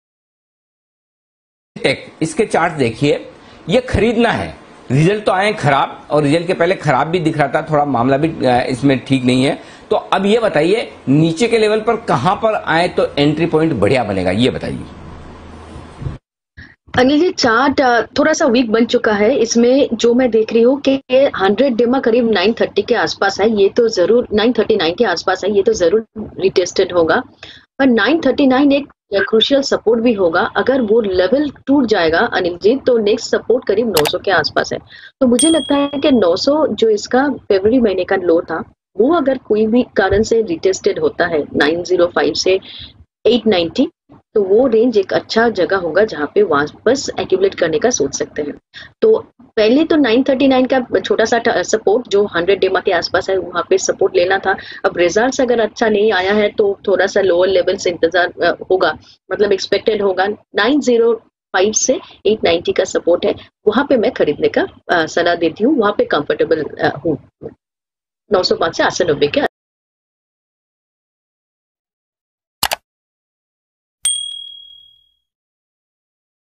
HCL tech, खरीदें या बेंच दें ?। Hcl tech share price । Hcl tech share tomorrow| Target 1200
IN THIS VIDEO I WILL TALK ABOUT Hcl tech STOCK AND IMPORTANT LEVELS & TARGET & INTRADAY & MUCH MORE , MUST WATCH. JOIN.
HCL Technologies is an Indian multinational information technology services and consulting company, headquartered in Noida, Uttar Pradesh, India. It is a subsidiary of HCL Enterprise.